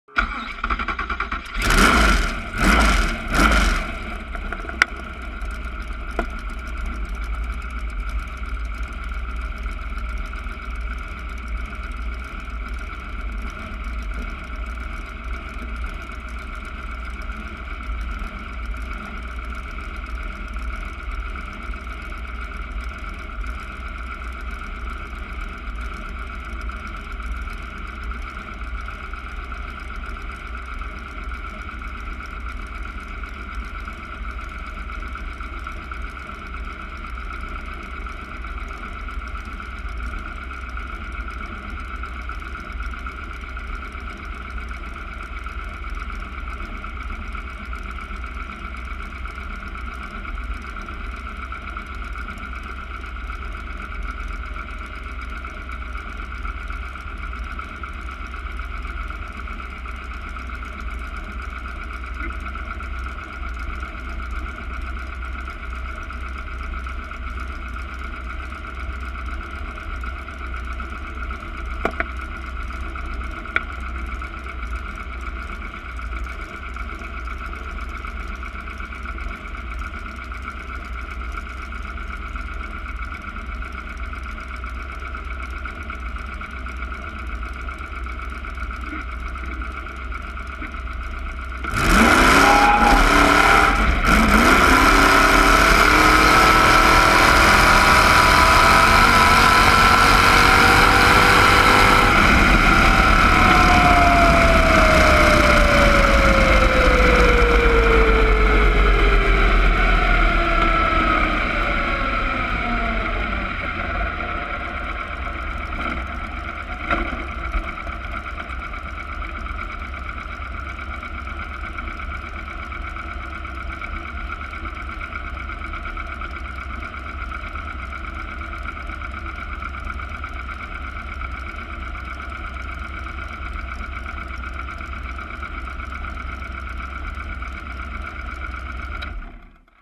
shakespeare county raceway drag car rear view parachute opening 2014 - Fed Slingshot
This video is about shackey drag car rear view parra